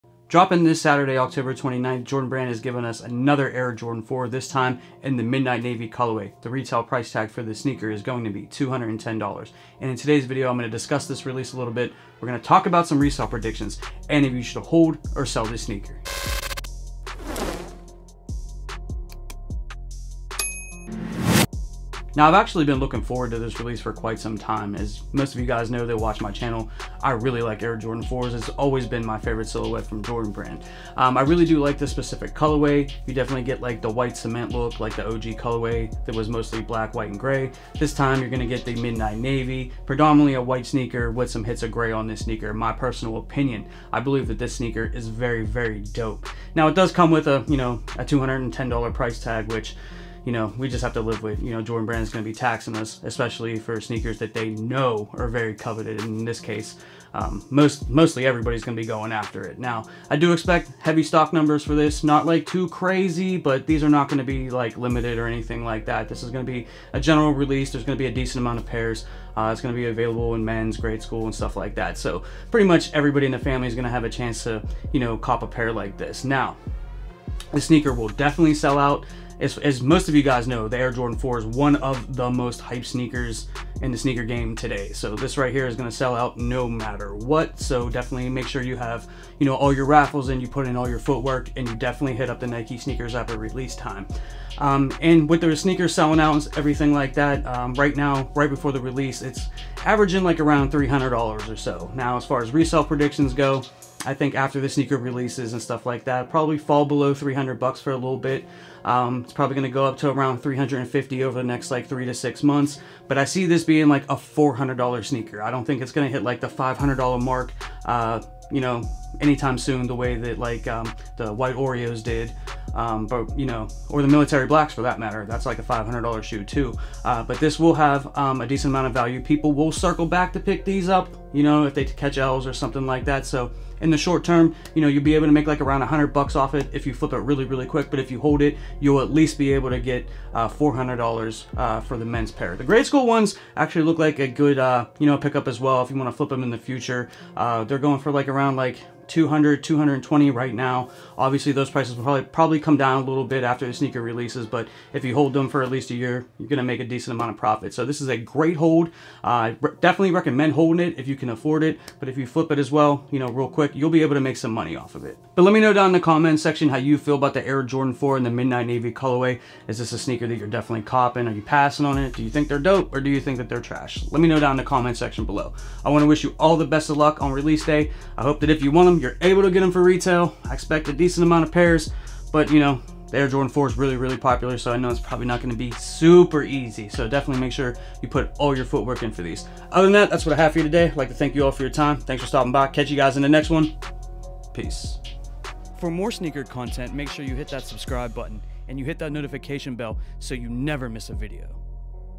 Air Jordan 4 “Midnight Navy” (Hold Or Sell)
In this video I discuss the release for the
Air Jordan 4 “Midnight Navy” and also provide some resale advice.

To see some of my sneaker reviews please click the link to enter the playlist